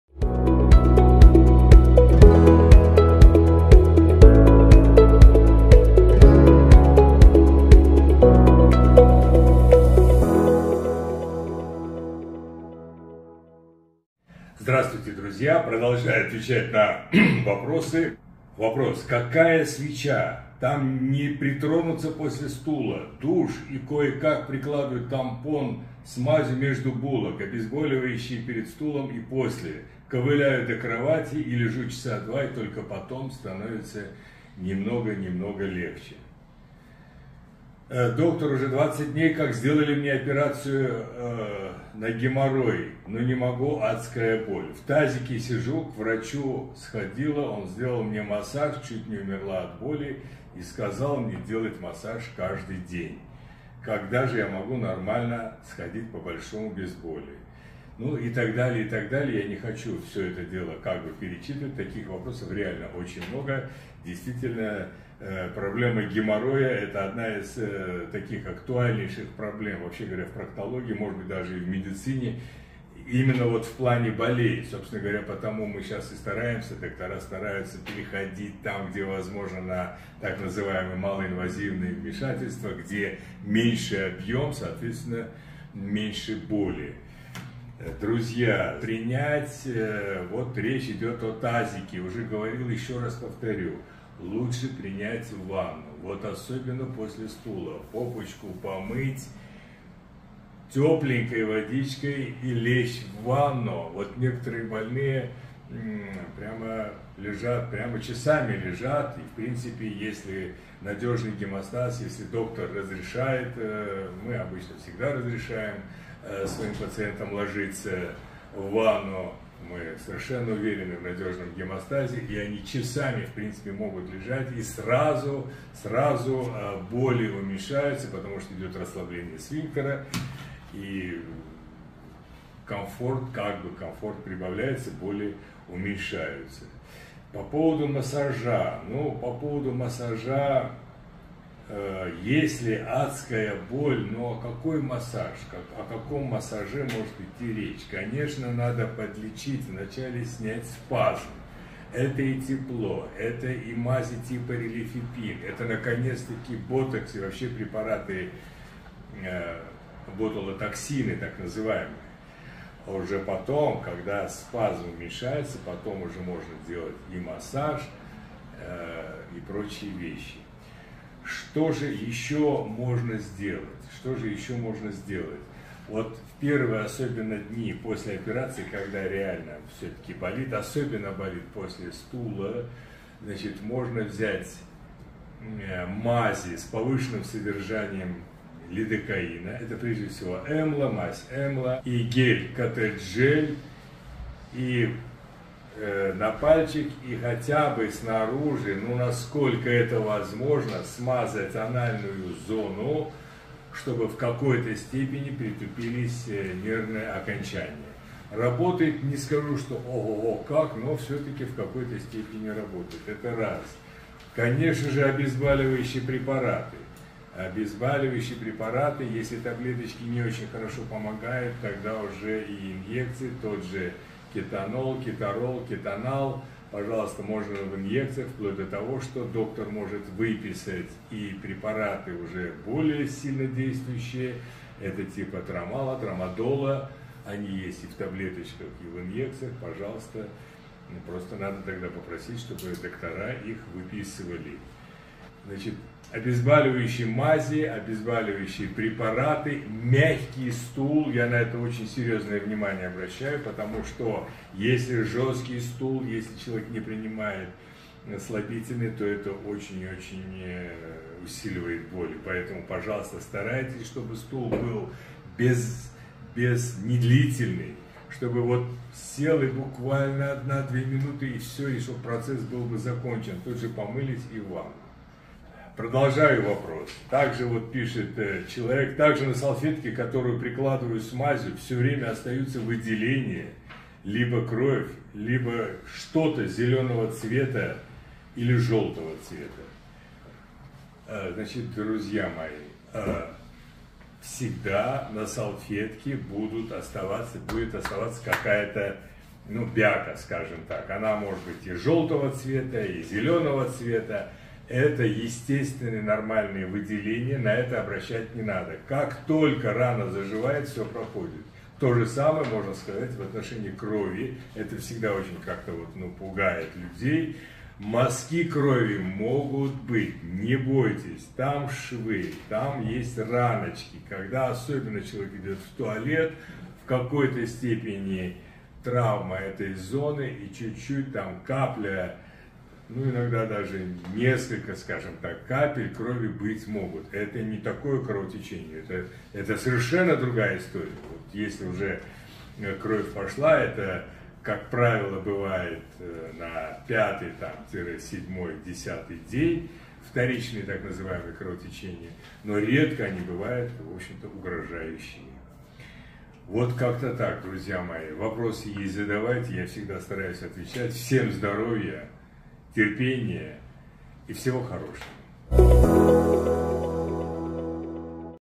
Еще раз повторю! ЛУЧШЕ ПРИНЯТЬ ВАННУ!
Вопрос " Какая свеча ? Не притронуться после стула! Душ, и кое как прикладываю тампон... обезболивающие перед стулом и после? Ковыляю до кровати и только потом становится немного , немного легче"

Доктор? Уже 20 дней как сделали мне операцию на геморрое, но не могу... адская боль, в тазике сижу, к врачу сходила, он сделал мне массаж, чуть не умерла от боли и сказал делать мне массаж каждый день. Когда же я могу нормально сходить по большому без боли? и.т.д.

Отвечает врач - Лев Карапетович Багдасарян, хирург-колопроктолог, врач высшей категории, кандидат медицинских наук, член ассоциации колопроктологов России, опыт более 40 лет.

Задавайте свои вопросы на моем форуме, и я обязательно отвечу